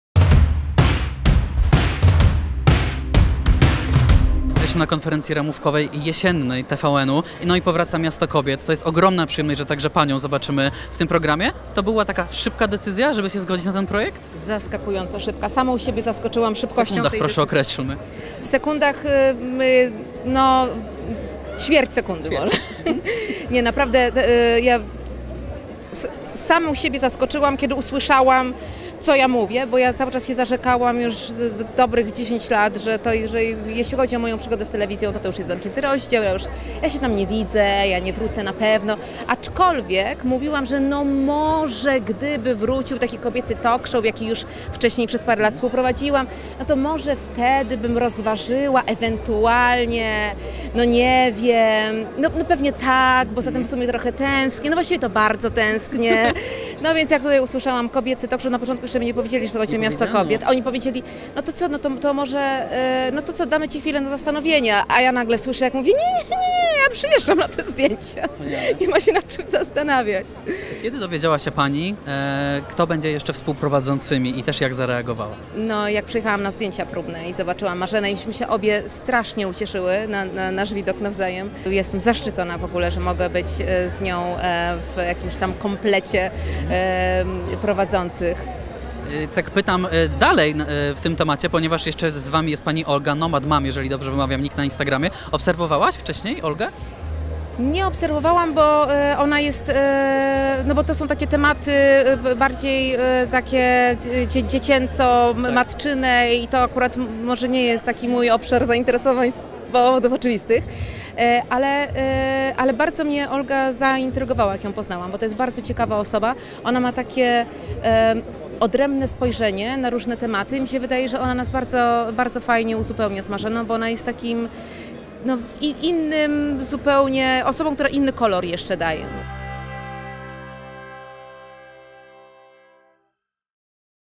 Aleksandra Kwaśniewska nie chciała tego 10 lat. TERAZ dała się przekonać
Ola Kwaśniewska  zarzekała się, że nie chce wracać do pracy przed kamerą, ale od września zobaczymy ją w TVN Style. Co przekonało ją do poprowadzenia Miasta kobiet? Pytał Rafał Kowalski